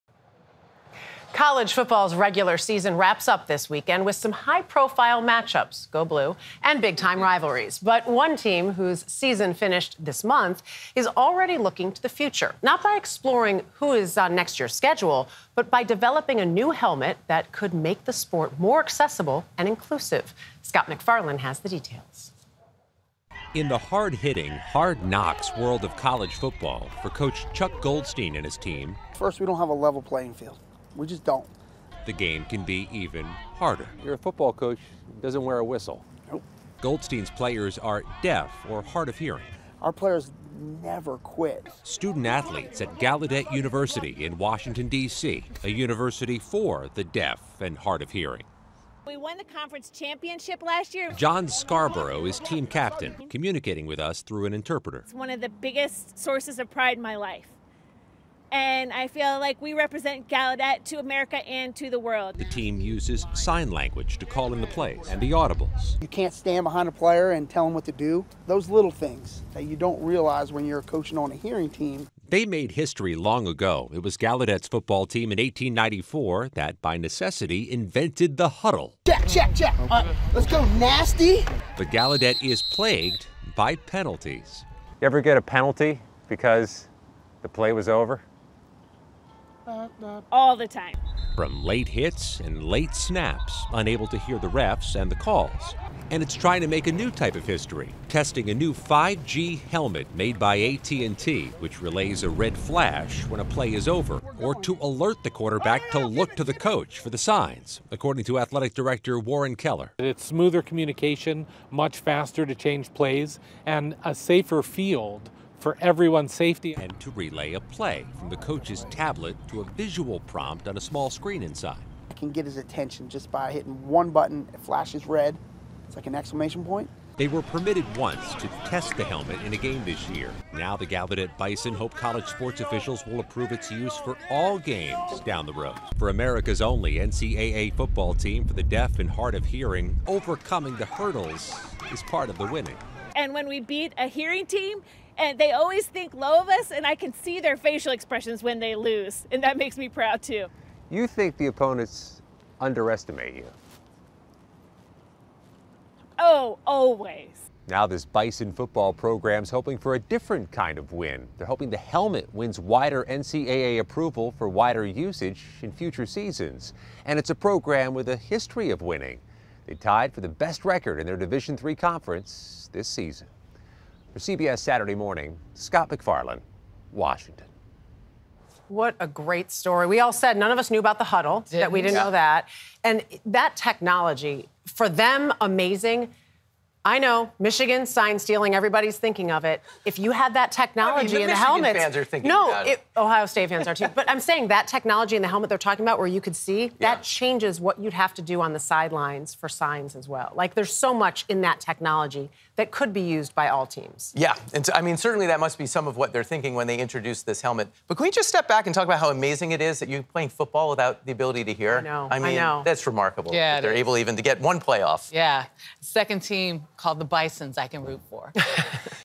New high-tech helmets make a difference for deaf or hard-of-hearing football players
For the football team at Gallaudet University, the hard knocks of the sport can be even harder. The players, coached by Chuck Goldstein, are all deaf or hard-of-hearing, so the team works uniquely to communicate and play. Recently, they tested high-tech helmets from AT&T that circumvented some of their challenges. Scott MacFarlane reports.